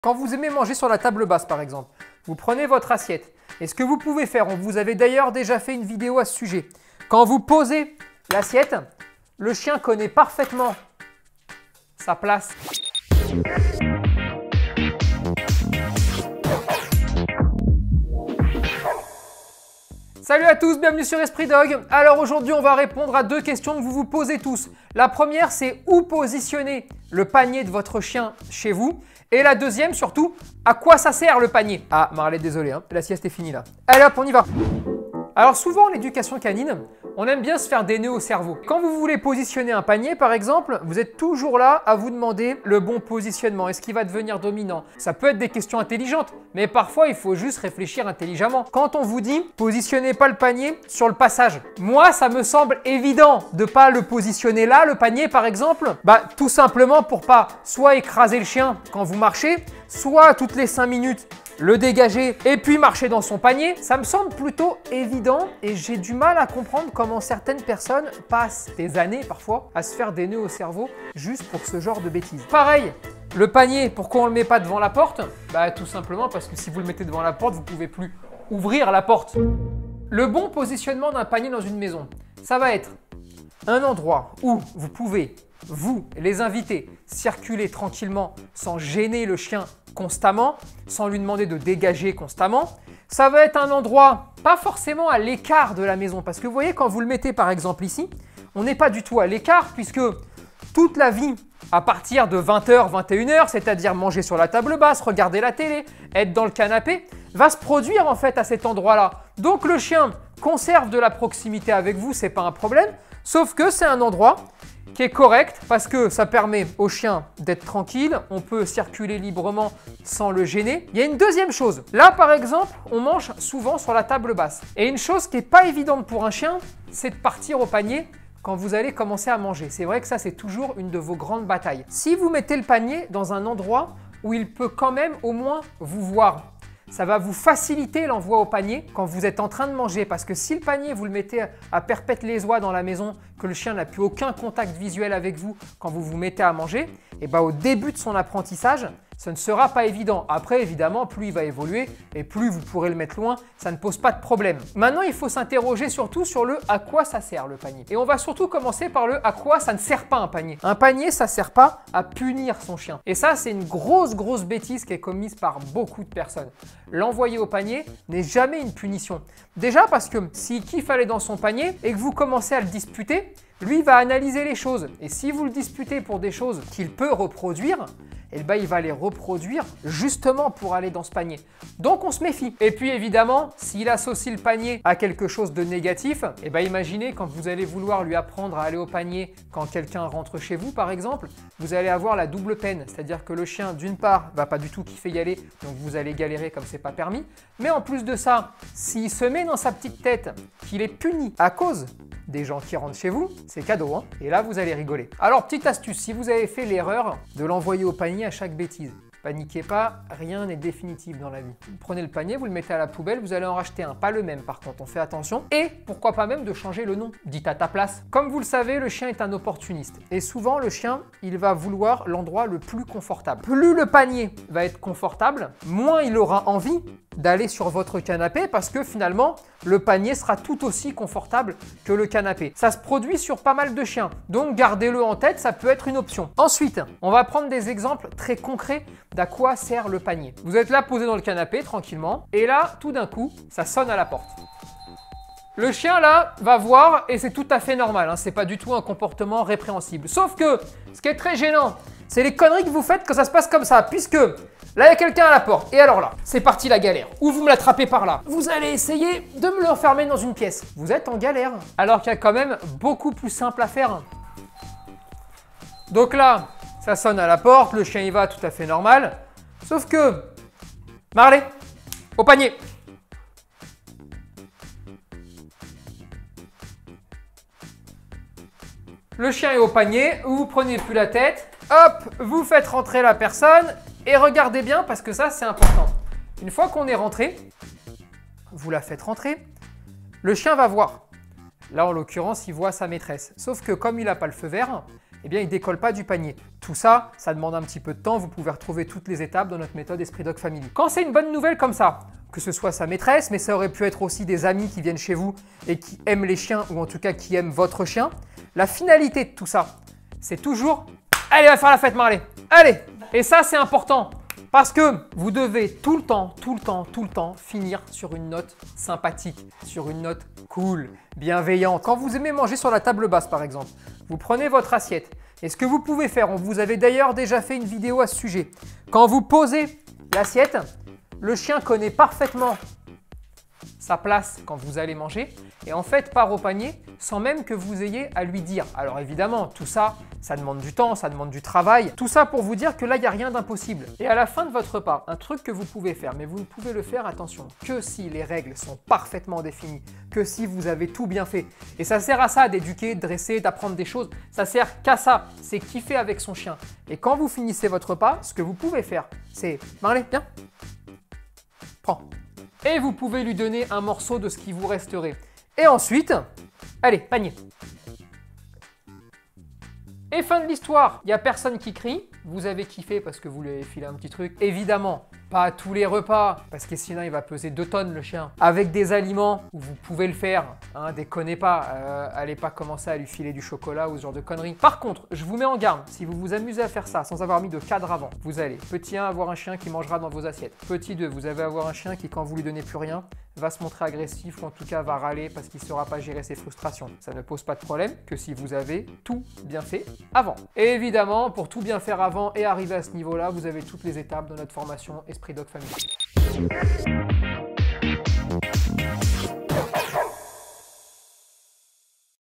Dresser un chien pour lui apprendre l'ordre "au panier" : pourquoi ?
Vous êtes nombreux à me poser la question : "Quelle est la meilleure place pour le panier de mon chien?"
Dans cette vidéo, nous allons vous dire quelle est la meilleure place chez vous pour le panier de votre chien.
Dresser un chien pour lui apprendre l'ordre "au panier" est souvent un ordre utilisé n'importe comment ou mis de côté, alors qu'il est indispensable pour une vie sans cri, sans stress !